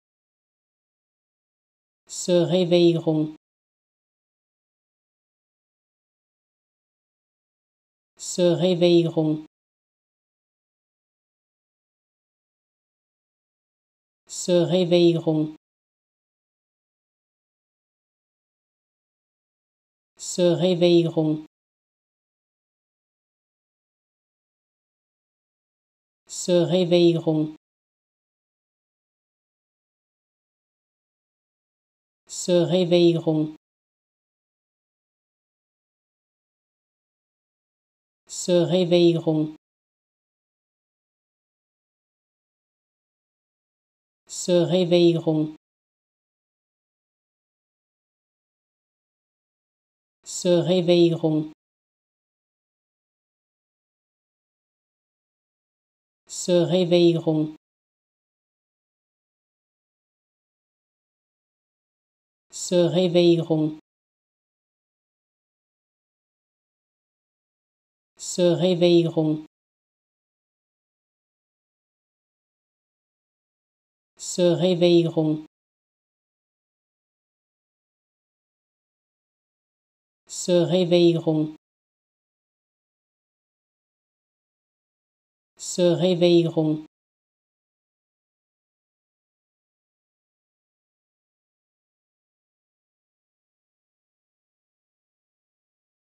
How to Pronounce ''Se réveilleront'' (To will wake up) Correctly in French
Learn how to say and properly pronounce ''Se réveilleront'' in French with this short pronunciation tutorial.